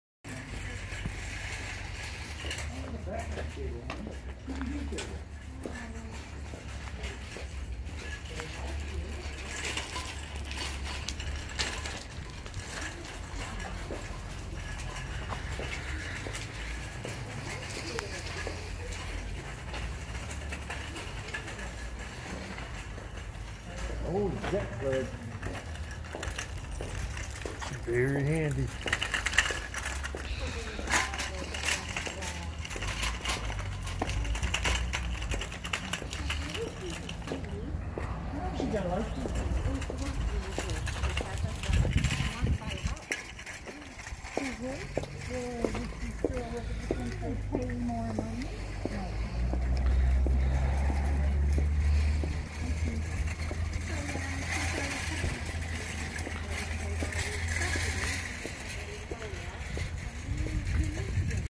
Pinconning Michigan Northwoods wholesale outlet 25 January 2021
A cool store with a huge selection of stuff